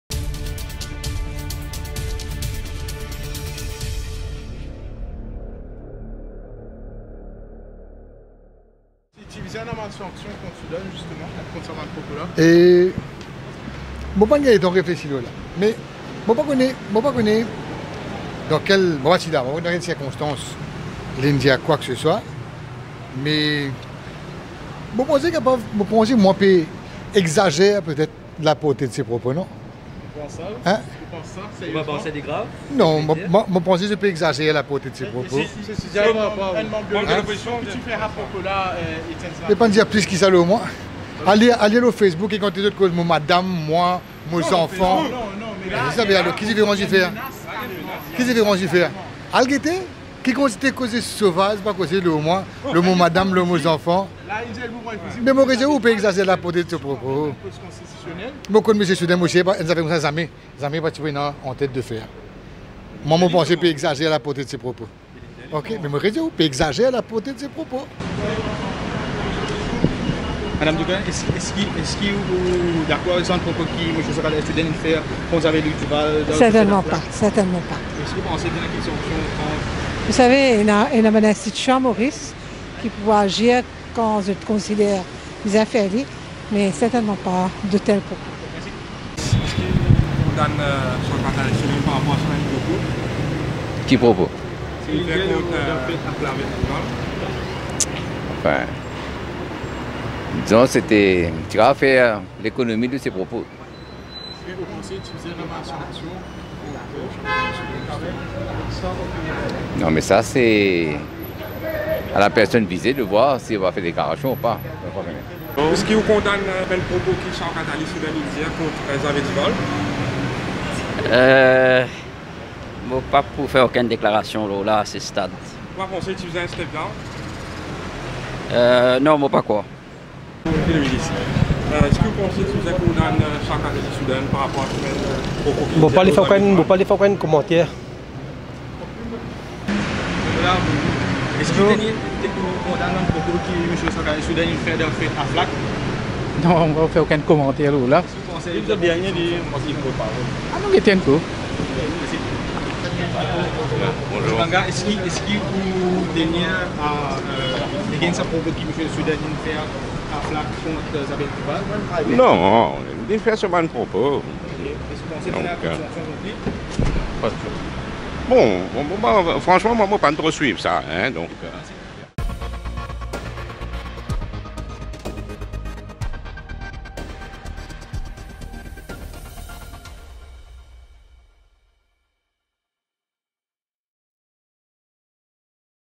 [Jihad de Soodhun] Sinatambou le défend, Dookun condamne, d’autres ministres esquivent
sSi le porte-parole du gouvernement dit ne pas avoir entendu les propos de Showkutally Soodhun, il affirme toutefois que les médias «exagèrent» le fait que celui-ci a affirmé vouloir tuer Xavier Duval. Leela Devi Dookun-Luchoomun est toutefois moins indulgente, affirmant que les propos de Soodhun ne sont pas excusables et qu’il a eu tort de les prononcer.
Si d’autres ministres, notamment Anil Gayan ou Ashit Gungah, restent loin de la polémique, ils avouent à demi-mot que le vice-Premier ministre aurait pu éviter ce genre de propos.
Ce mardi, lors d’une fête culturelle à Flacq, Showkutally Soodhun a affirmé que dans un accès de colère, il aurait pu prendre le revolver de son garde du corps pour tirer une balle sur Xavier Duval au Parlement.